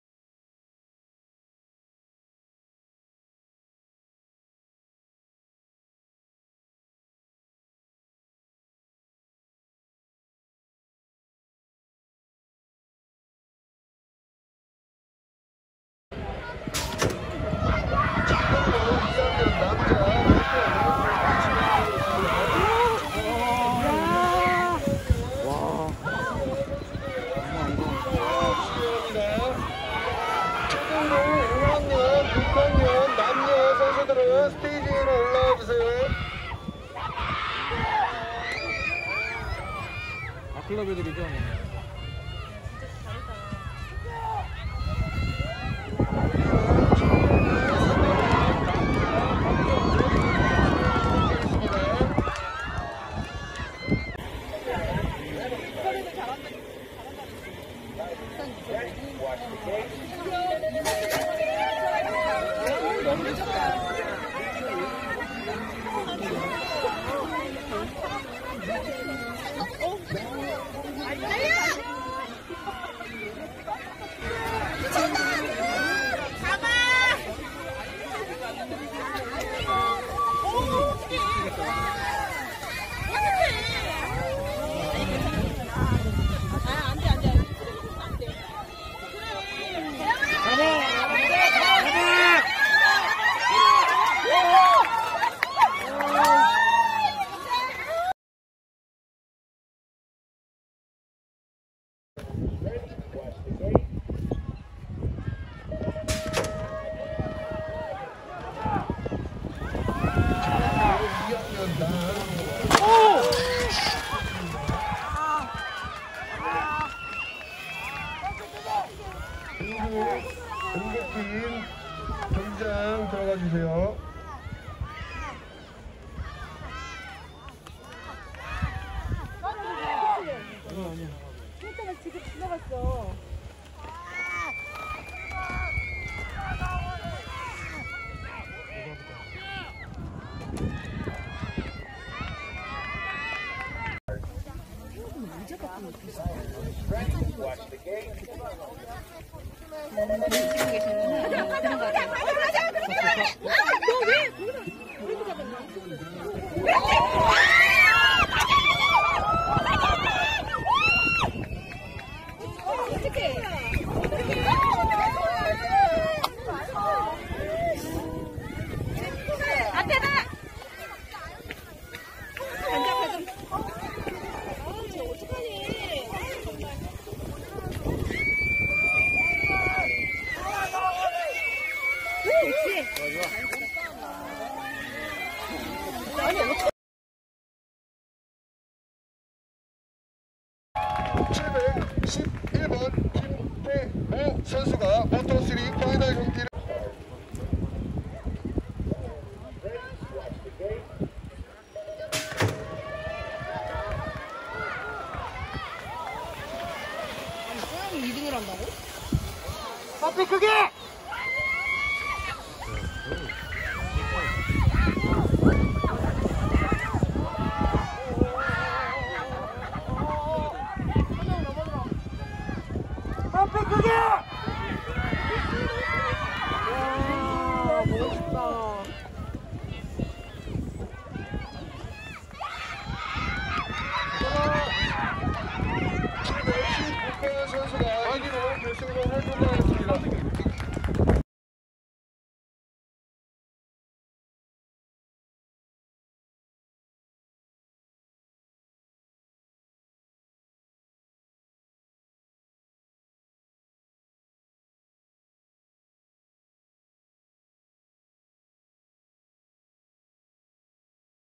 20th June 2021 Yangyang BMX Race 전국선수권대회 UCI CN
Korea kids BMX. I have been creating these films that would be used for my children history that they would have a reference currently or when they are grown up.

Kimtaeyoung Instagram : tae.young0919
Kimtaewoo instagram: kimtaewoo.bmx